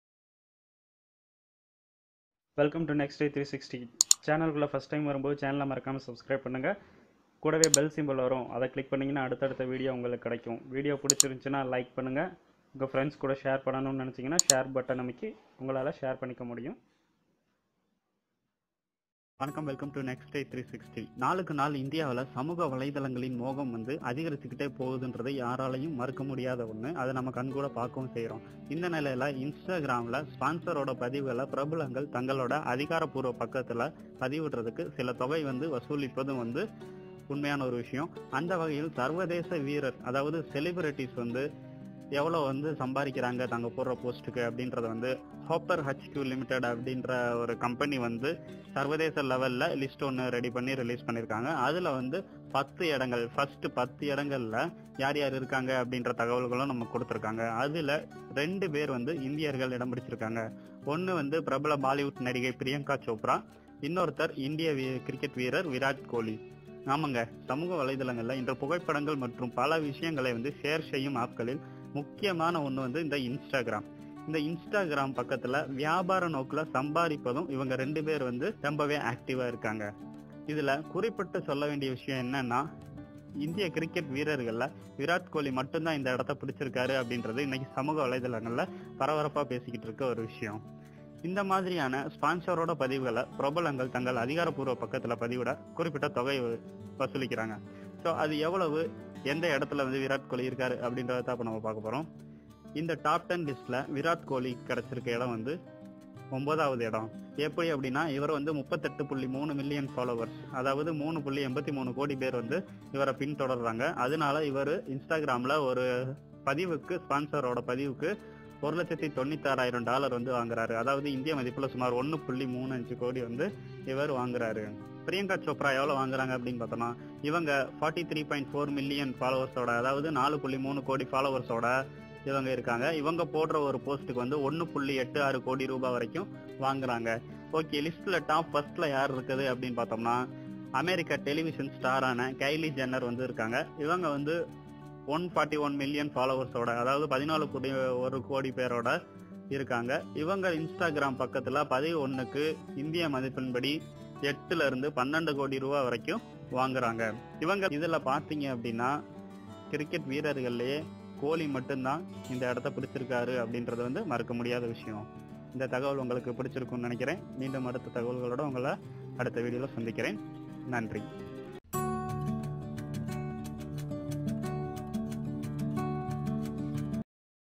இன்ஸ்டாகிராம் பதிவு மூலம் ரூ.1.35கோடி சம்பதிக்கும் வீராட் கோலி! | Nextday360
இன்ஸ்டாகிராம் பதிவு மூலம் ரூ.1.35கோடி சம்பதிக்கும் வீராட் கோலி! இப்படியும் கூட சம்பாதிக்க முடியுமா..

நாளுக்கு நாள் இந்தியாவில் சமூக வலைதளங்களின் மோகம் அதிகரித்துக் கொண்டே தான் வருகிறது என்பதை நாம் கண்கூடாக பார்க்க முடிகிறது. இந்த நிலையில் இன்ஸ்டாகிராமில் ஸ்பான்சர்களின் பதிவுகளை பிரபலங்கள், தங்களின் அதிகாரப்பூர்வ பக்கத்தில் பதிவிட தொகை வசூலிக்கப்பது வழக்கம். அந்த வகையில் சர்வதேச வீரர் செலிப்பிரிட்டு அதிகளவு இதன் மூலம் சம்பாதிக்கிறார்கள் என்ற பட்டியல் வெளியிட்டப்பட்டுள்ளது.
Hopper Hq Ltd இந்த பட்டியலில் முதல் 10 இடங்களில் உள்ள, இந்தியர்கள் இருவர் தானாம். ஆமாங்க ஒன்று பிரபல பாவிவுட் நடிகை பிரியங்கா சோப்ராவும், இதில் இடம் பெற்றுள்ள மற்றொரு இந்தியர், கிரிக்கெட் வீரர் வீராட் கோலியாம்.

ஆமாங்க.. சமூக வலைத்தளங்களில் இன்று புகைப்படங்கள் மற்றும் பல விதமான விஷயங்களை ஷேர் செய்யும் ஆப்களில் முக்கியமான ஒன்றாக கருதப்படுவது இன்ஸ்டாகிராம் சமூக வலைத்தளம். இந்த இன்ஸ்டாகிராம் பக்கத்தில் அதிகளவில் வியாபார நோக்கில் சம்பாதிப்பதும் இவர்கள் இருவர் தானாம்.

இதில் குறிப்பிட்டு சொல்ல வேண்டிய விஷயம் என்னவெனில் கிரிகெட் வீரர்களில் ஒருவர் தான் இந்த இடத்தை பிடித்துள்ளராம். அந்த வகையில் வீராட் கோலி தான் இந்த இடத்தை பிடித்திருப்பது சமூக வலைத்தளங்களில் மிகப் பரப்பரப்பாக பேசப்படுகிறது.

இன்றைய நாளில் ஸ்மார்ட்போன் இல்லாதவர்கள் யாரும் இல்லாமல் இல்லை. அப்படி இருக்கையில் இது போன்ற சமூக வலைத்தளங்கள் இளைஞர்கள் மத்தியில் பிரபலாமாக்கப்பட்டு வருவது பெரிய விஷயம் இல்லை என்றாலும் ,இதை வைத்தும் இவ்வளவு சம்பாதிக்க முடியுமா? என்றும் கேள்வி எழுகிறது. குறிப்பாக திரை பிரபலங்கள், முக்கிய பிரமுகர்கள், விளையாட்டு வீரர்கள் ஆகியோர் இன்ஸ்டாகிராம் பக்கத்தில்தான் தங்கள் சுவாரஸ்யமான புகைப்படங்களை பகிர்கின்றனர்.

இந்த மாதிரியான ஸ்பான்சர்களின் பதிவுகளை பிரபலங்கள், தங்கள் அதிகாரப்பூர்வ பக்கத்தில் பதிவிட, குறிப்பிட்ட தொகை வசூலிக்கப்படுவது வழக்கம்.

இந்த வகையில் கடந்த 2019ம் ஆண்டிற்கான இன்ஸ்டாகிராமில் பணக்காரர் யார்? என்ற ஆய்வு மேற்கொள்ளப்பட்டது. இந்த ஆய்வின் முடிவில், விளையாட்டு வீரர்களில் ஸ்பான்சர் பதிவுகள் மூலம் யார் அதிக பணம் சம்பாதிக்கிறார்கள் என்ற பட்டியலை வெளியிடப்பட்டுள்ளது.

இதில் உலக அளவில் முதல் 10 இடங்களில் உள்ள ஒரே ஒரு கிரிக்கெட் வீரர் விராட் கோலி தானாம். அதுவும் அவர் 9வது இடத்தில் இருக்கிறாராம். இதில் கவனிக்கப்பட வேண்டிய விஷயம் என்னவெனில் இன்ஸ்டாகிராமில்

9. Vitat koli இவரை சுமார்  38.3 million Followers = 3.83 crore  கோடி பேர் பின்தொடர்கின்றனராம்.

 இவரது இன்ஸ்டாகிராம் பக்கத்தில் ஸ்பான்சர் பதிவு ஒன்றுக்கு 1,96,000 டாலர்(இந்திய மதிப்பில் ரூ.1.35 கோடி பெறுகிறார்.

1. Kylie Jenner -  American television stars
141 million Followers = 14.1 crore

இவரது இன்ஸ்டாகிராம் பக்கத்தில் ஸ்பான்சர் பதிவு ஒன்றுக்கு 1.26million டாலர் per post (இந்திய மதிப்பில் ரூ.12.6 கோடி பெறுகிறார்.

2. இந்த பட்டியலில் போர்ச்சுகல் Portugal கால்பந்து வீரர் footballer player-  Cristiano Ronaldo  கிறிஸ்டியானோ ரொனால்டோ
இவரை சுமார்   177 million Followers = 17.7 crore கோடிபேர் பின்தொடர்கின்றனராம்.

 இவரது இன்ஸ்டாகிராம் பக்கத்தில் ஸ்பான்சர் பதிவு ஒன்றுக்கு 9,75,000 டாலர்(இந்திய மதிப்பில் ரூ.6.75 கோடி பெறுகிறார்.
முதல் இடத்தில் உள்ளார்.

3 வது இடத்தில் பிரேசில் Brazil கால்பந்து வீரர் நெய்மரும்,
123 million Followers = 12.3 crore கோடி

4 வது இடத்தில் அர்ஜென்டினா Argentina கால்பந்து வீரர் Lionel Messi லயோனல் மெஸ்சியும் உள்ளனர்.
126 million Followers = 12.6 crore கோடி

5 வது இடத்தில் England இங்கிலாந்து கால்பந்து வீரர் David Beckham டேவிட் பெக்காம் இருக்கிறார்.
57.5 million Followers = 5.75 crore கோடி

US singer Ariana Grande
160 million = 16 crore

television star Kim Kardashian
144 million = 14.4 crore

American singer Selena Gomez.
154 million = 15.4 crore

மேலும் இதுவே திரை பிரபலங்கள் மத்தியில் பிரியங்கா சோப்ரா
43.4 million = 4.34 crore கோடி
1.86 கோடி ரூபாய் பெறுகிறாராம்.